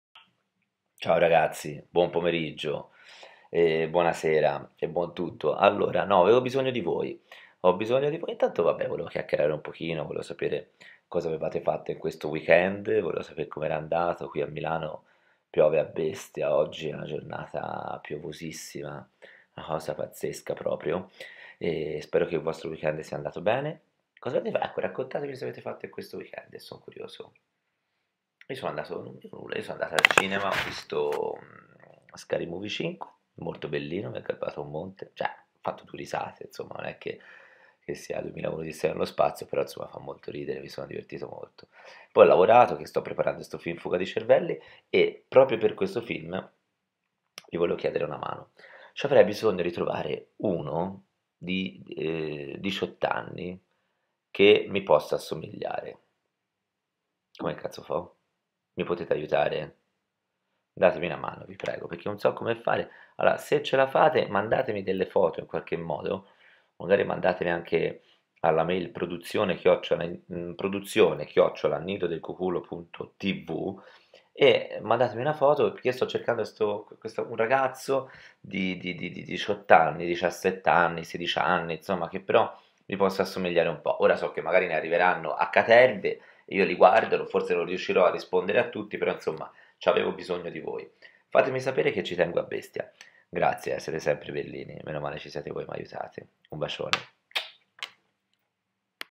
Paolo Ruffini - ricerca sosia diciottenne per il film Fuga di Cervelli
Cari amici, raccontatemi come avete passato il fine settimana, io sono stato a vedere Scary Movie 5 al cinema...a proposito di cinema: per il mio film Fuga di Cervelli sono alla ricerca di un mio sosia diciottenne! AIUTATEMI!!!